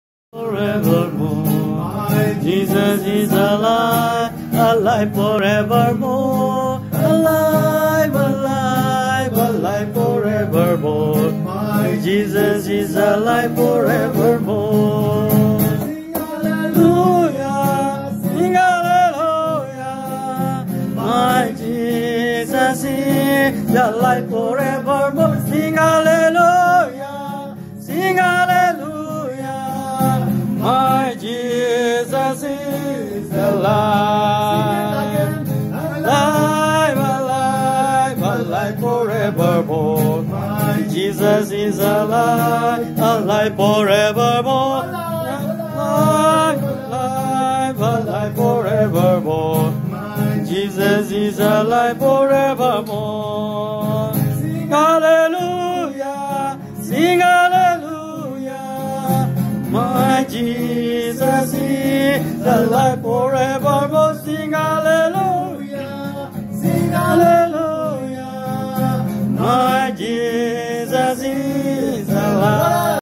Jamming in Jamaica 🇯🇲
Jamming with some of the MOP brothers in Saint Ann’s Bay, Jamaica 🇯🇲